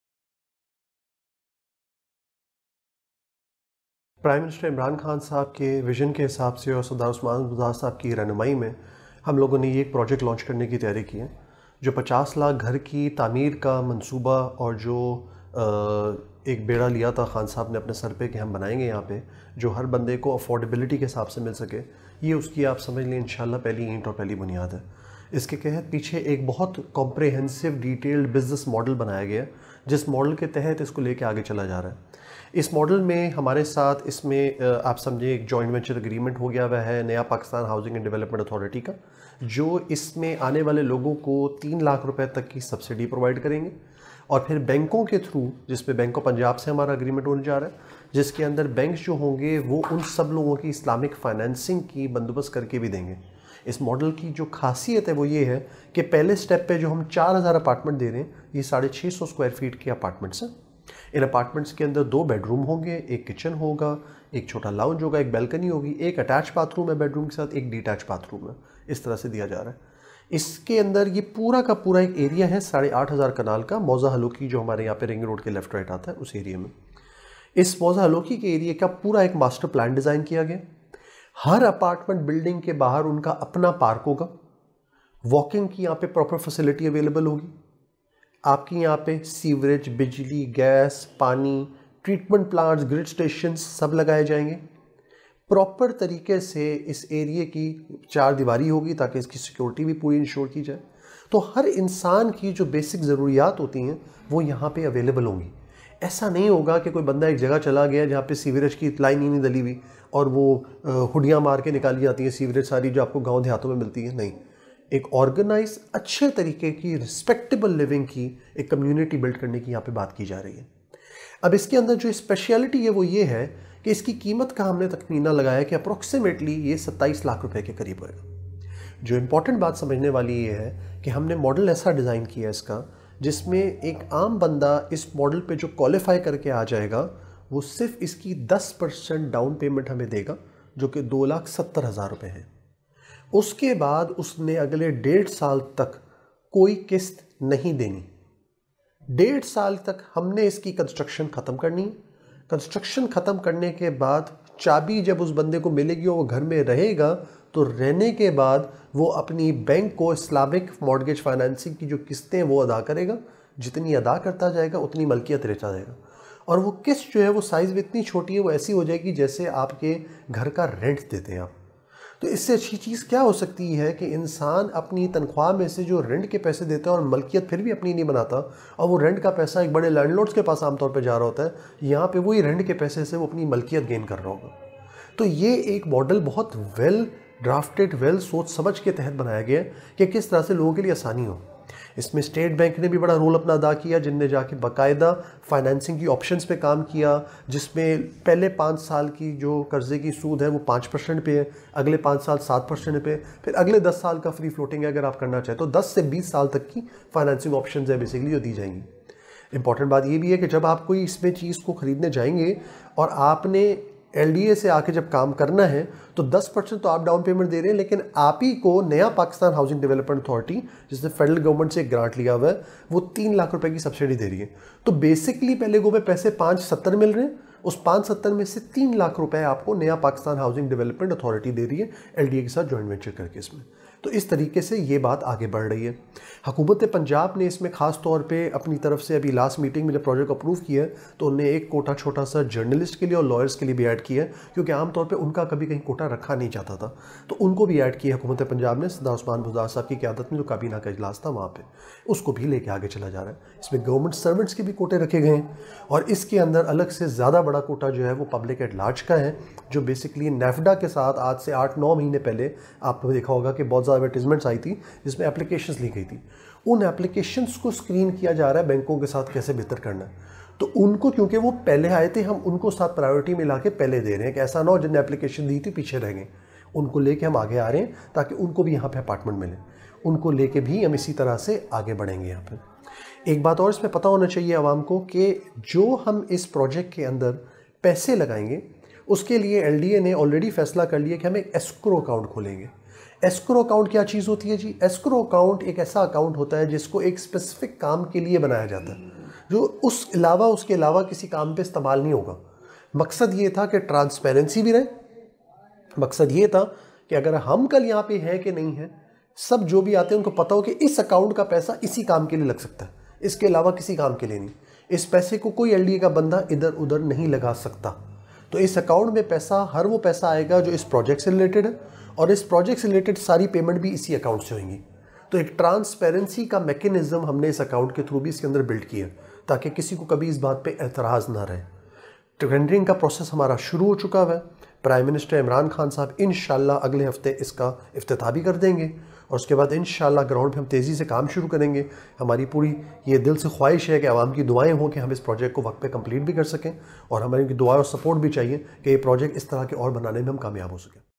4,000 apartments to prove first brick in the construction of 5mn homes' dream of PM Imran: SM Munir
LAHORE-- Construction of 4,000 apartments to prove first brick in the construction of 5 million homes' dream of Prime Minister Imran Khan: SM Munir